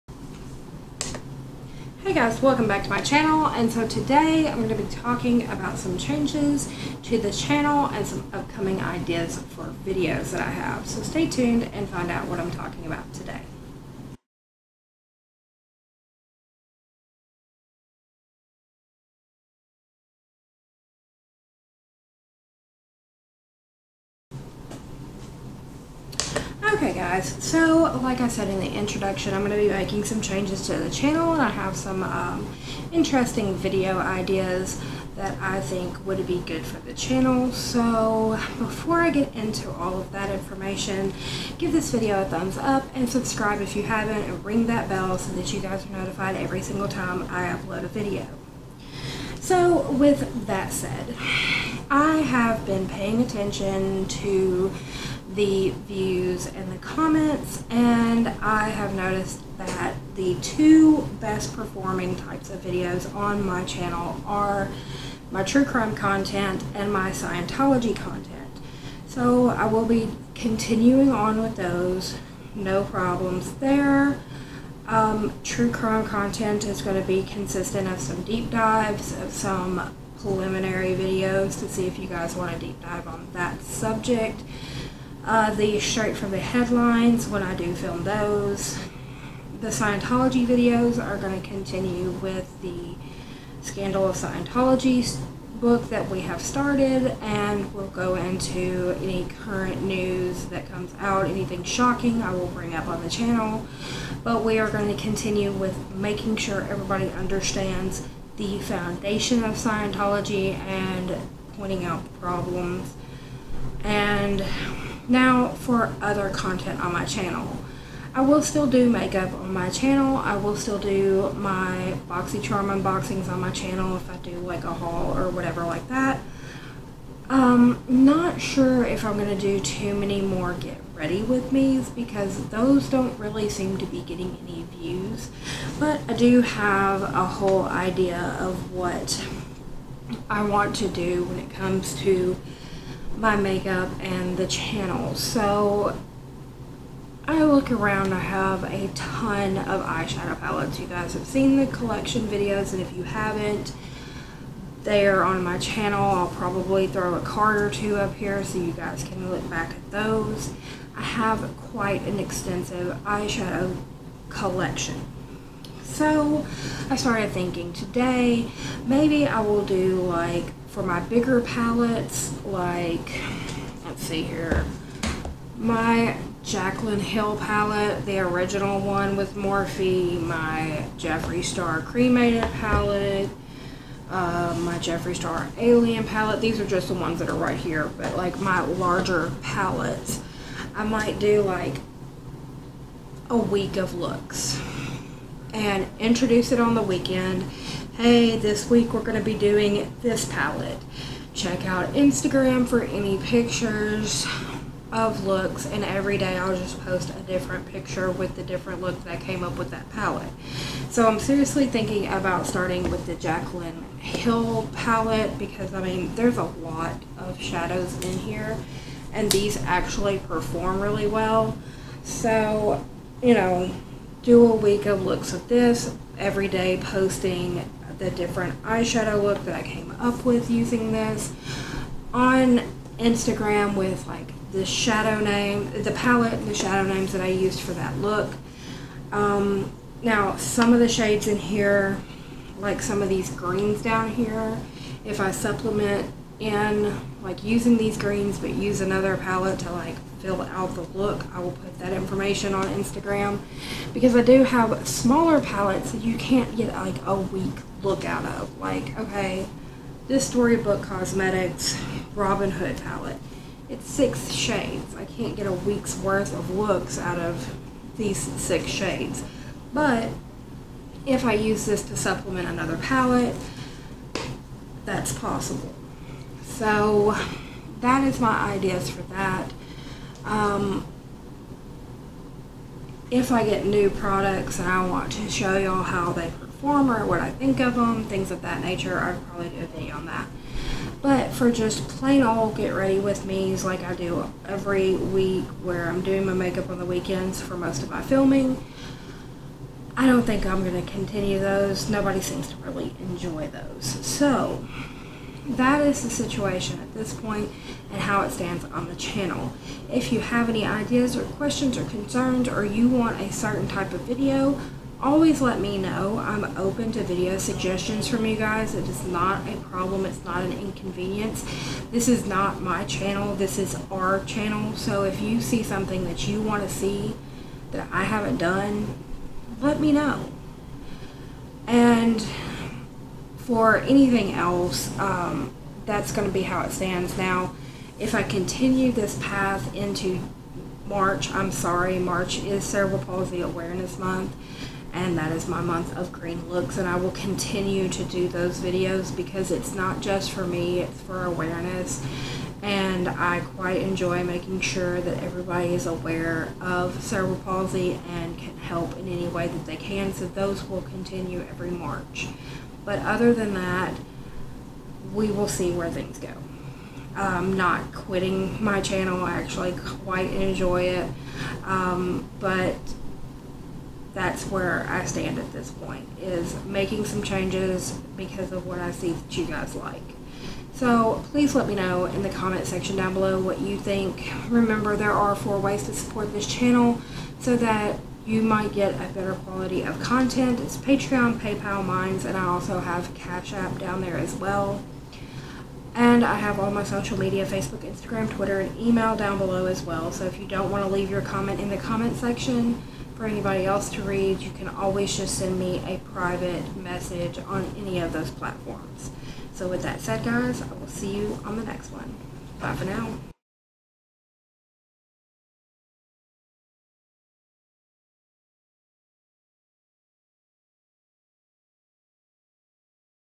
Changes to the Channel
Hey guys! In today's video, I am discussing changes that I am making to the channel content. These changes are based off the interaction and the views on certain videos. I want to bring the content that you guys want, so there are changes that are coming. Let me know what you think of the video and the content!